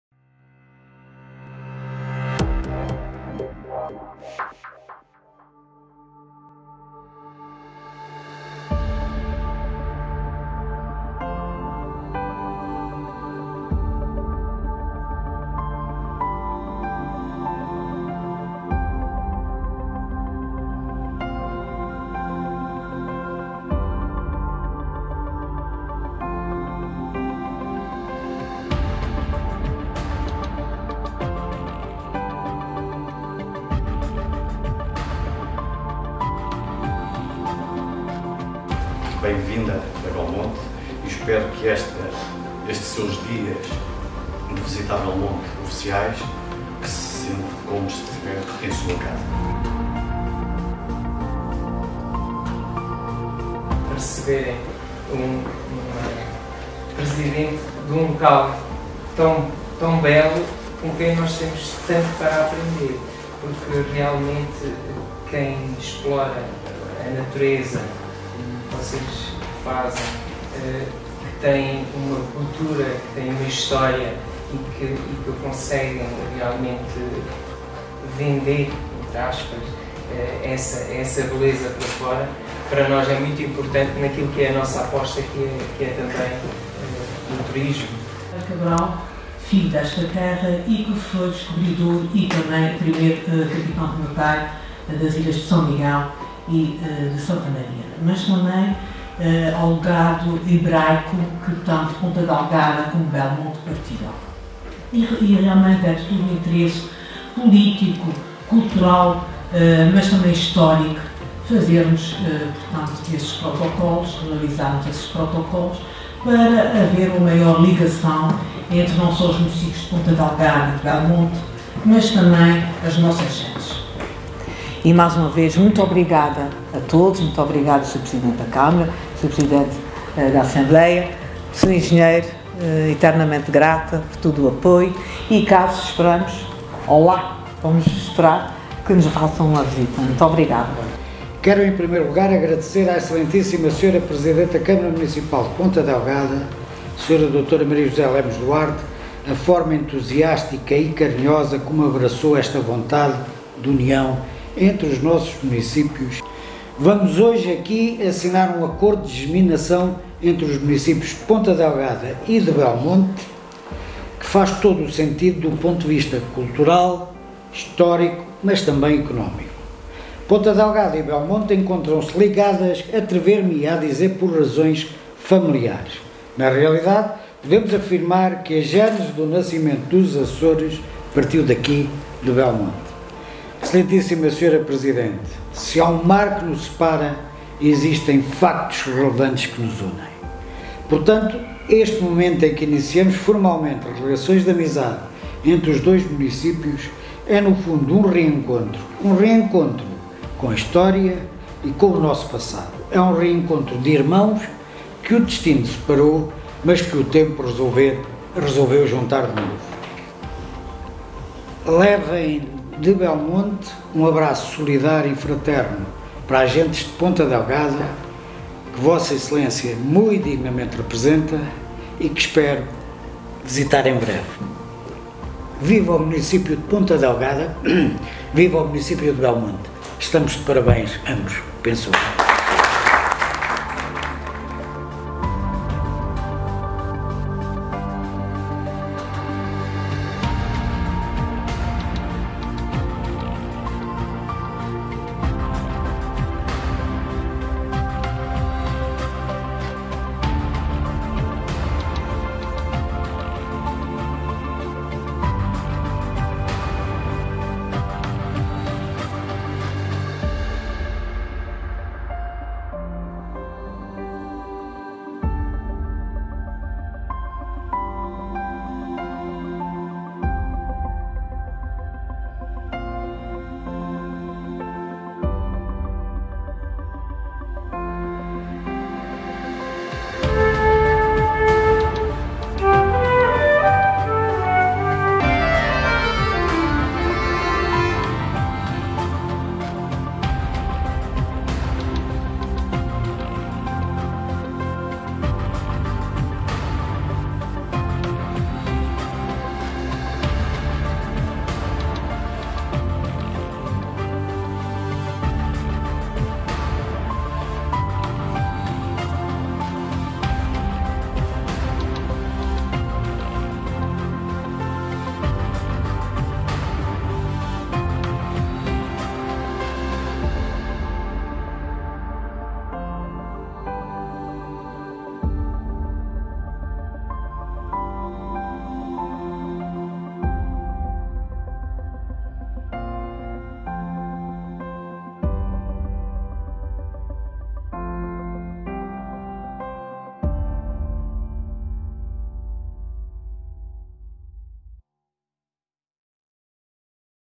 BELMONTE - PONTA DELGADA | Protocolo de Geminação e Cooperação
Assinatura de Protocolo de Geminação entre os Municípios de Belmonte e Ponta Delgada, e Protocolo de Cooperação entre o Museu Judaico de Belmonte e a Antiga Sinagoga de Ponta Delgada.